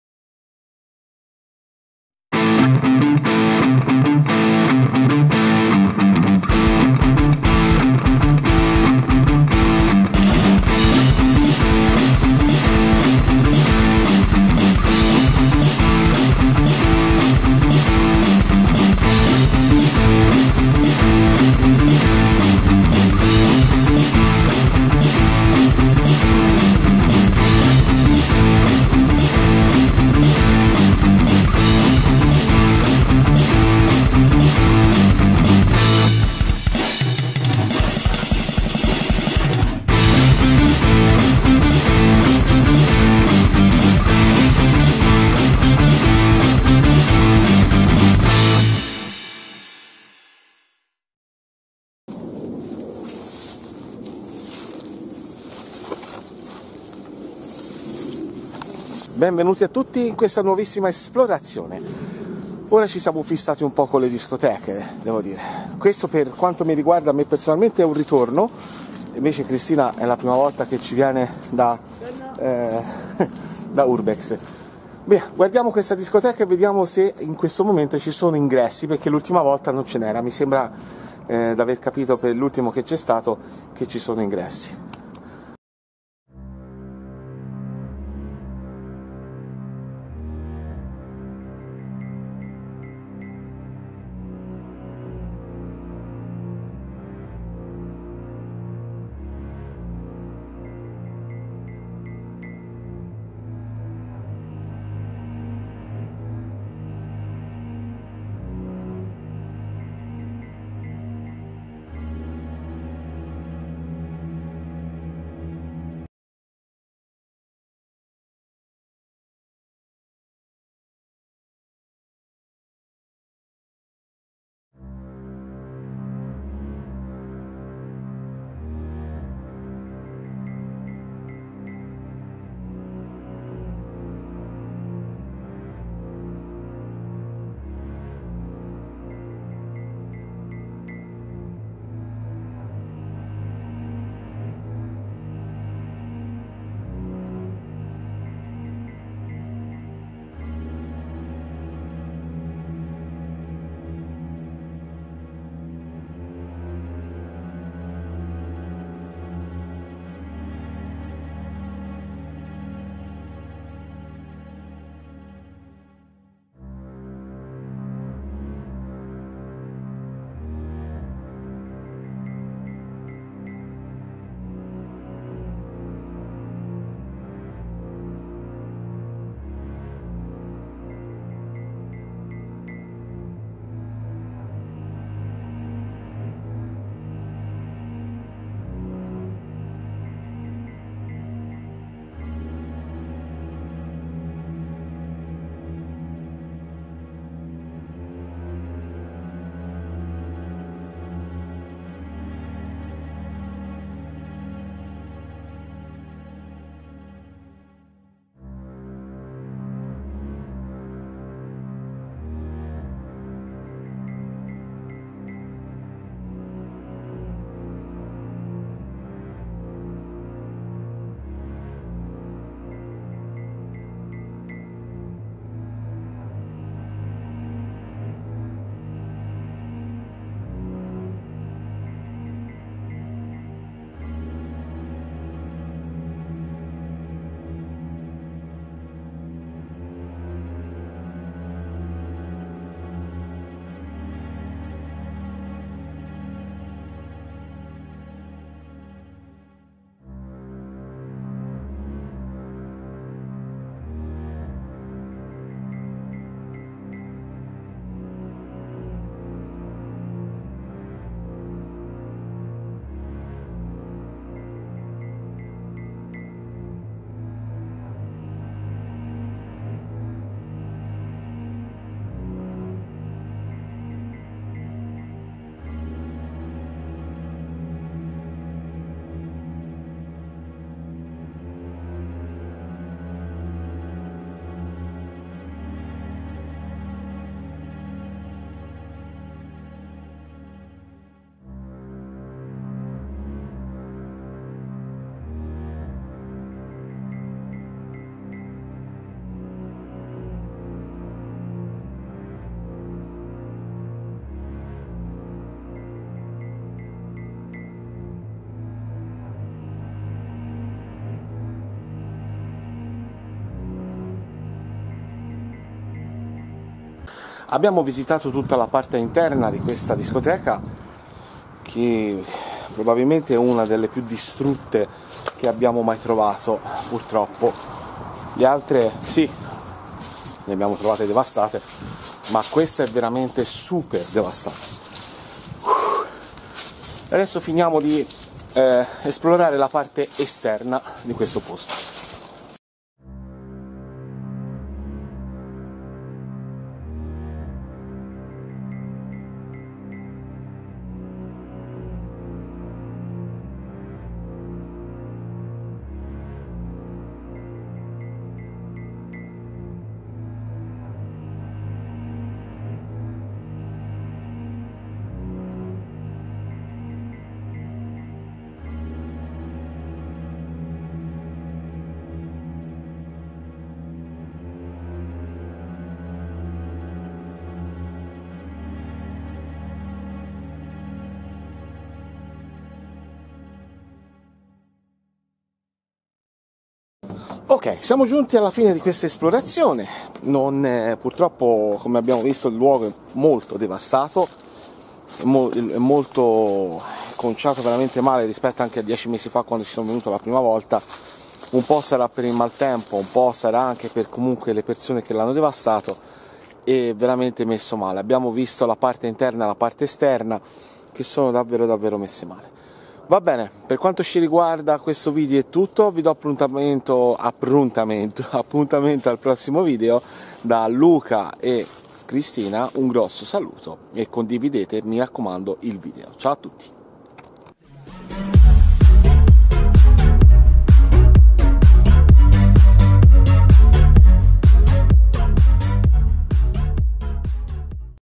La discoteca abbandonata (urbex italia)
Ritorno a una delle ex discoteche più attive nel litorale toscano negli anni 90. La devastazione totale.